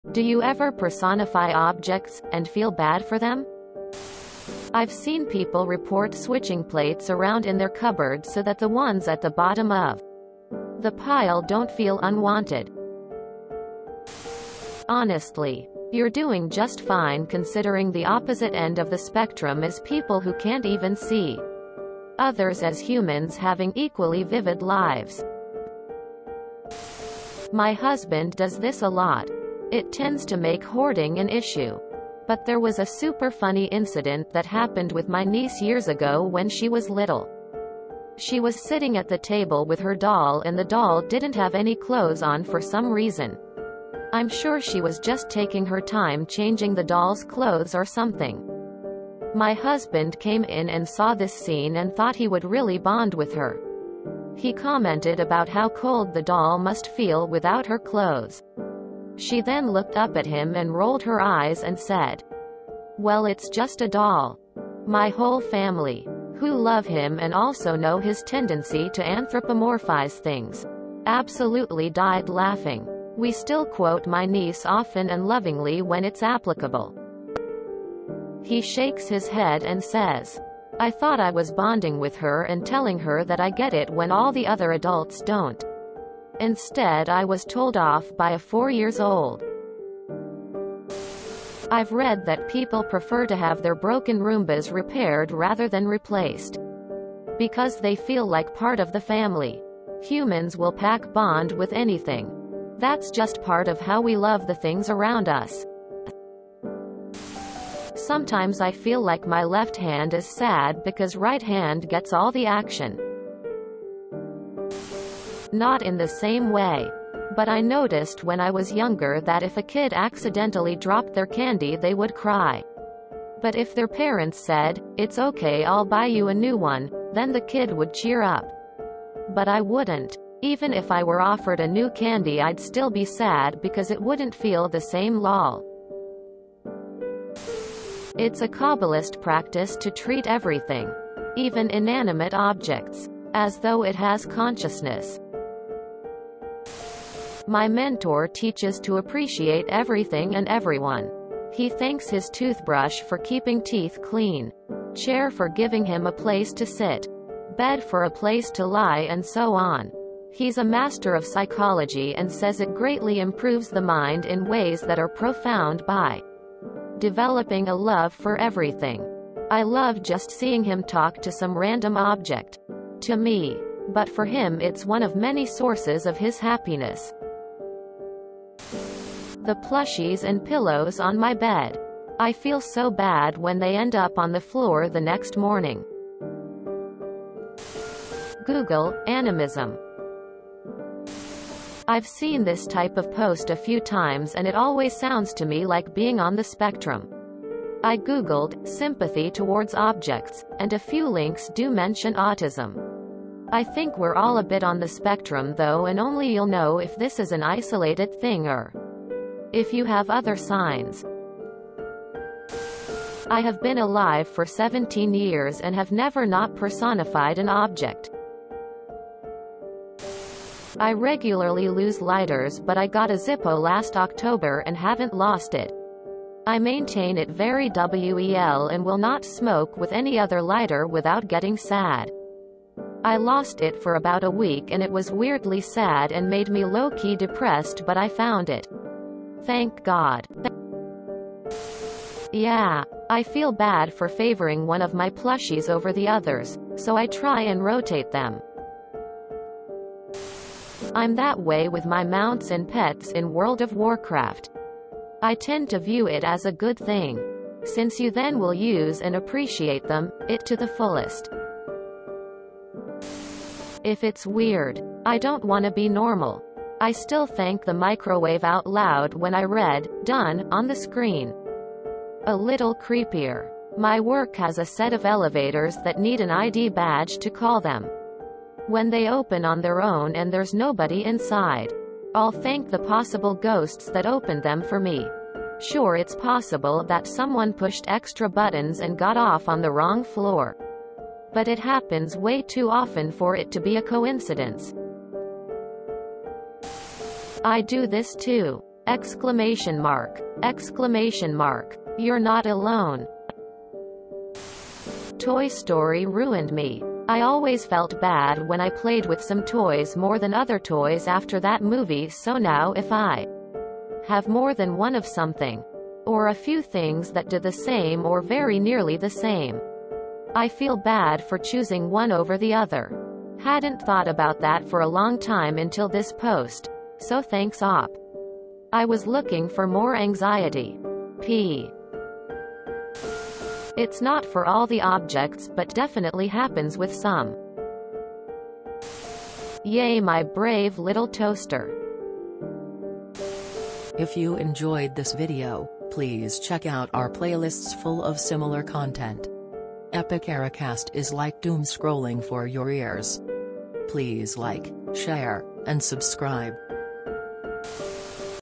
Do you ever personify objects, and feel bad for them?
Do you ever personify objects, and feel bad for them? (r/TooAfraidToAsk)

Source: read-it-throwaway on Reddit (link removed because YouTube keeps dinging me for linking to a "site with nudity"!?)